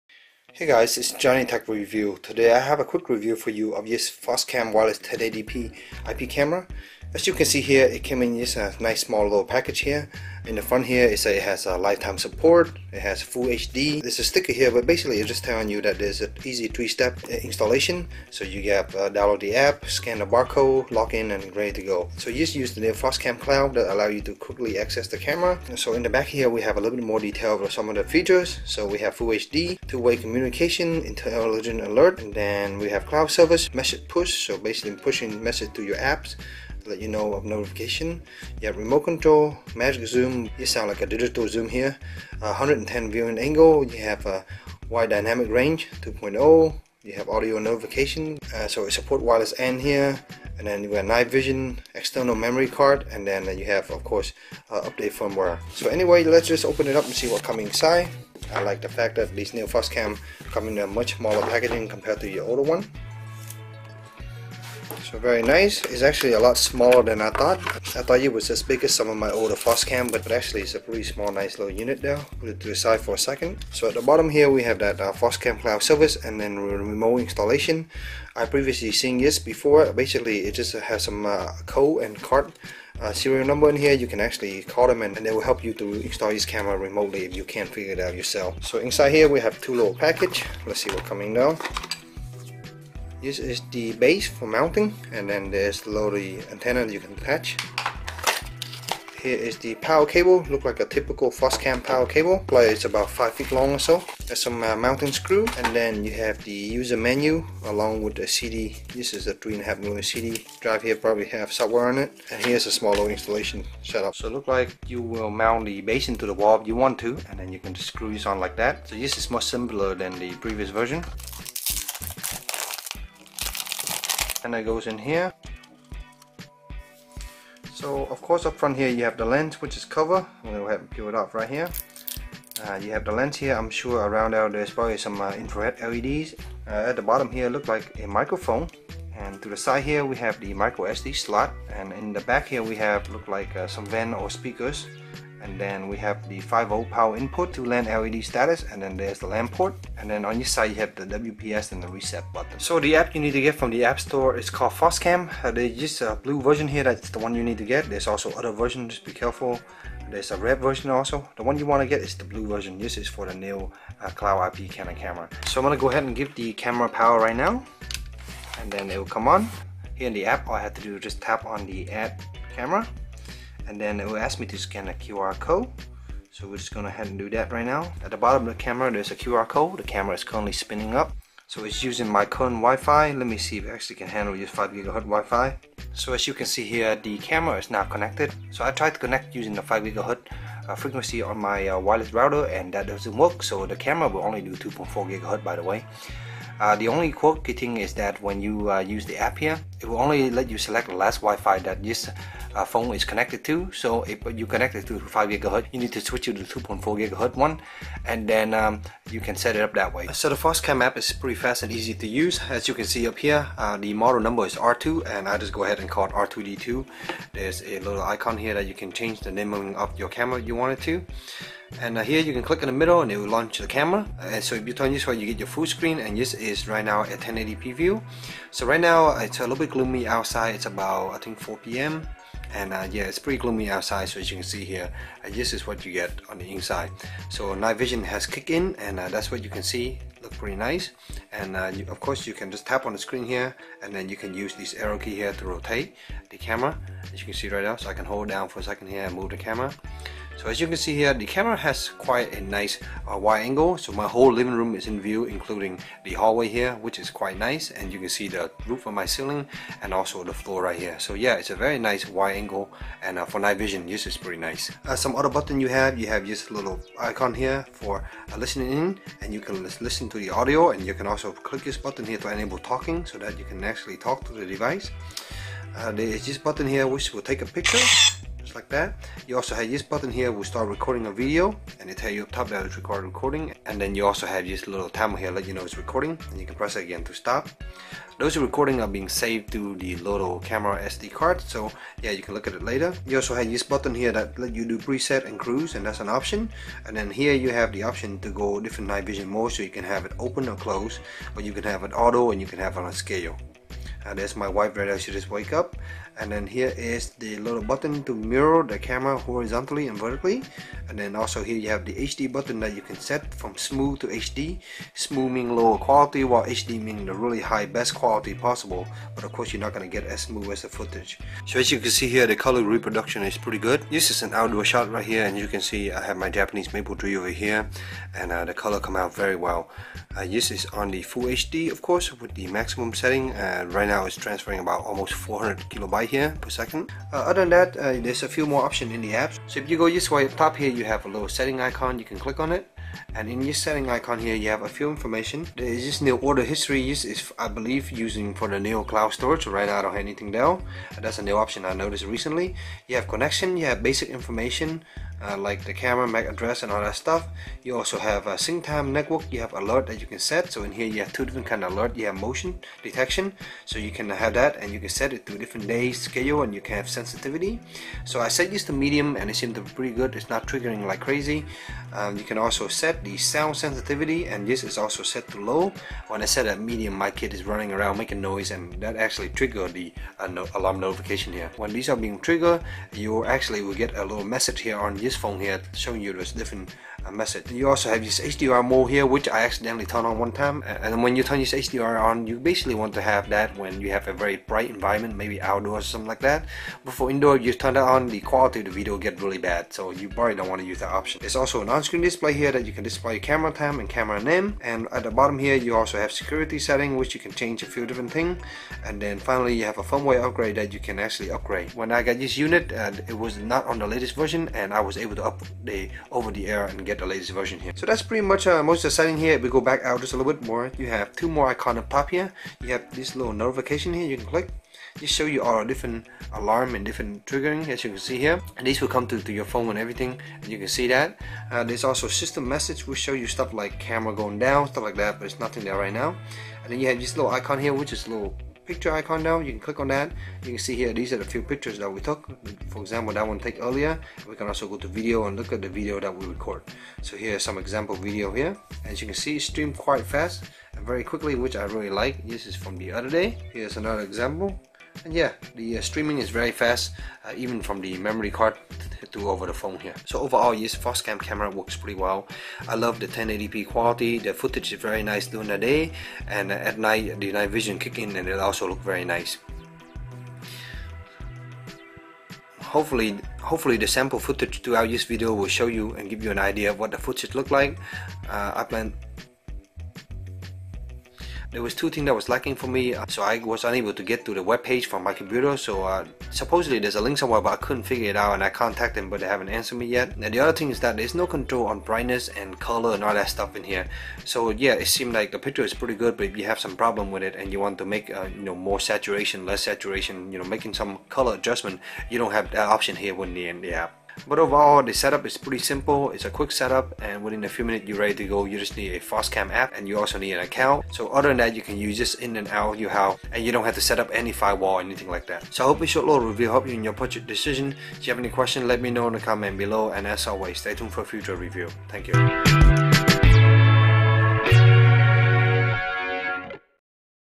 Indepth Foscam R2W IP Security Camera REVIEW
Please watch my video for the full review:
The Good:
 - 1080p quality of the video is nice and clear, color are saturated
 - Night vision is good 10-15 feet still visible
 - Pan, tilt, zoom is smooth easy to operate. I wish there's swipe option so you don't have to open up the remote control
 - The app is easy to use and provide plenty of setting option.
 - Cloud storage is an option for only 1 cent a month currently.
The Bad:
 - The myfoscam.com website use an outdate plugin which doesn't work on chrome and IE 64.
 - The video and picture are stored in some weird database on the SD card, cannot just easily download them. The app provides no way to save it either
 - The lack of color/brightness control on the app is annoying